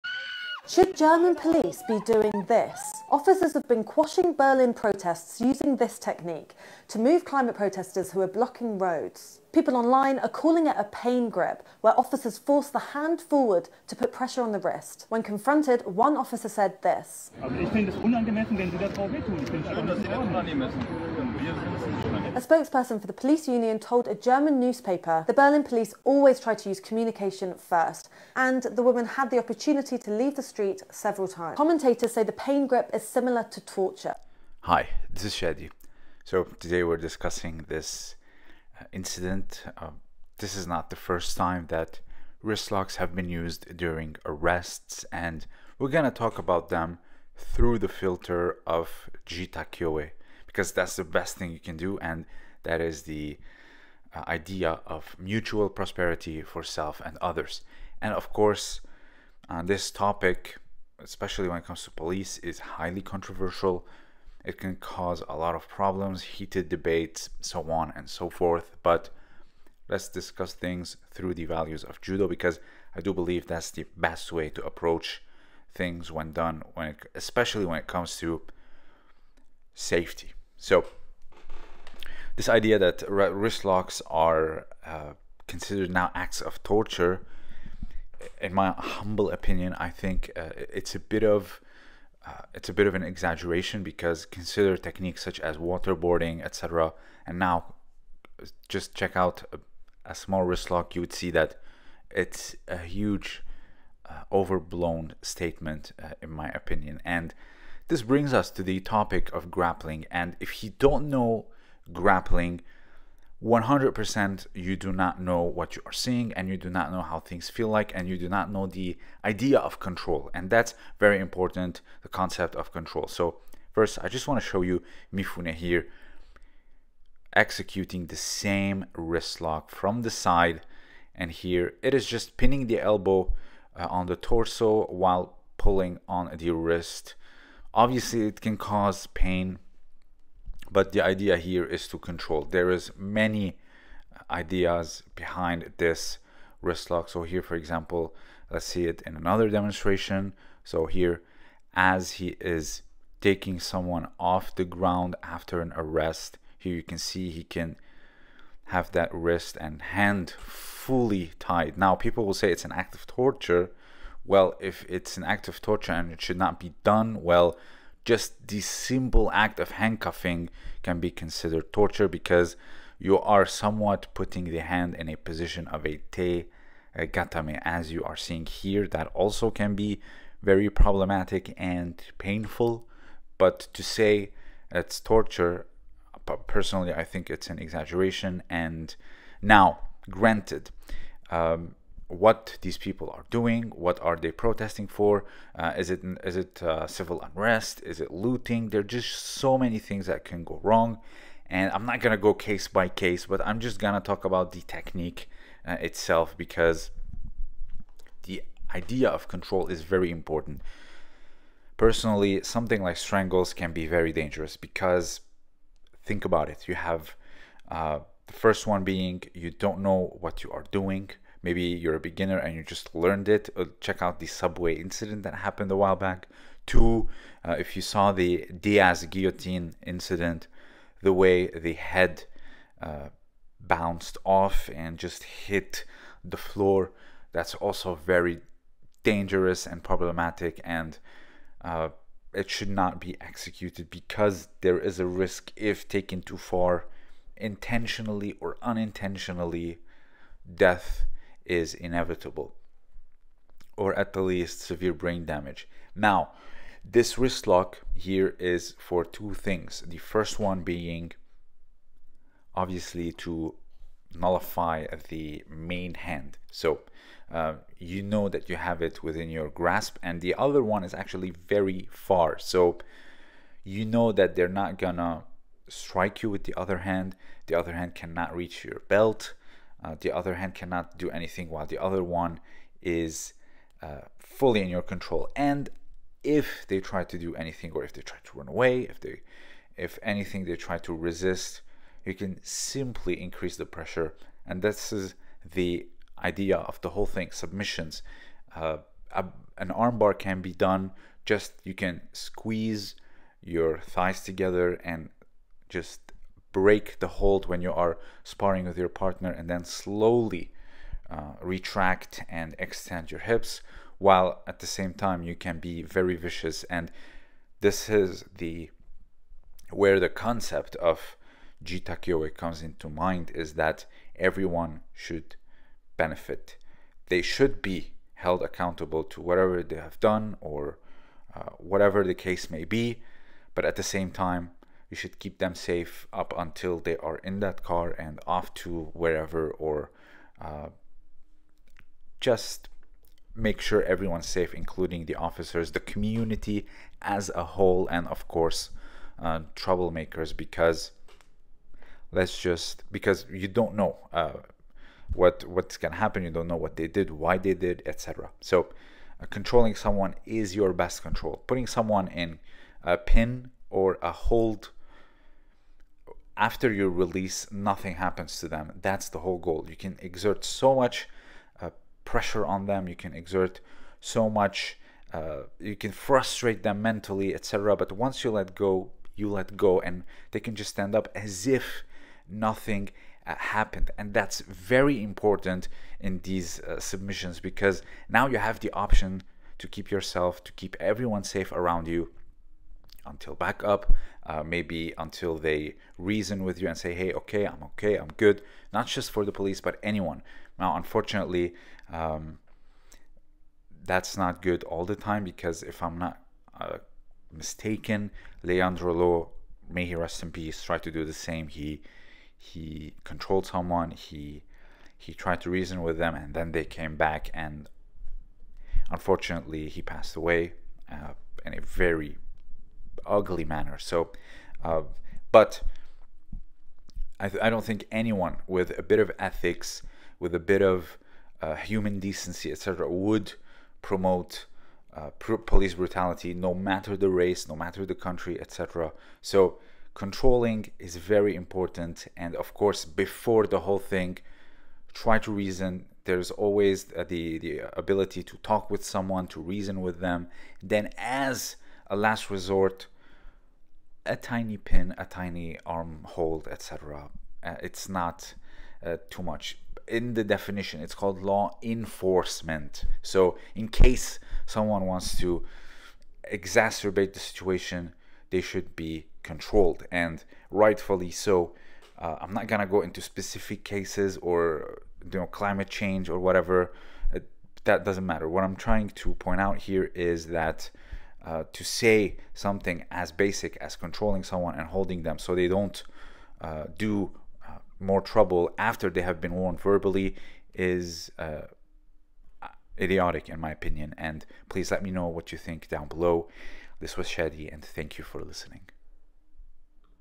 Wrist locking techniques during arrests are now considered torture?!?!
This video discusses the grappling techniques during arrests and whether or not they are ethical.

My book "Une silhouette de mots":